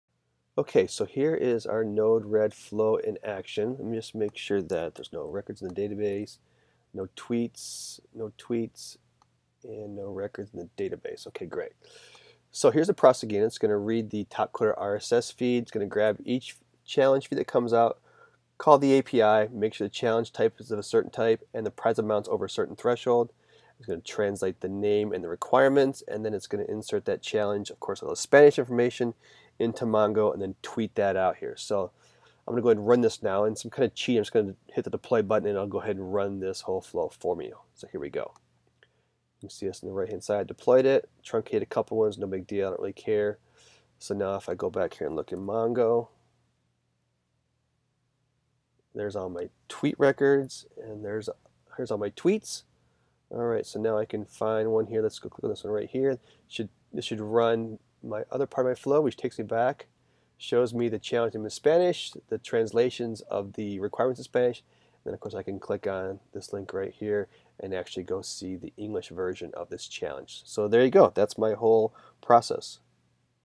Node-RED for Topcoder Challenges with IBM Watson & Bluemix
Simple flow that reads the topcoder RSS feed, translates the challenge name and requirements into Spanish (using Watson), persists the data to MongoDB and then tweets them challenges in Spanish.